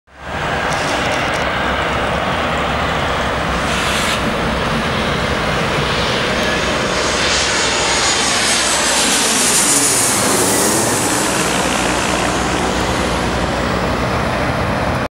EMBRAER E-190 de Austral Lineas Aéreas aterrizando en AEP 07-09-2016
Video tomado el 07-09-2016 en aeroparque Jorge Newbery de la ciudad de Buenos Aires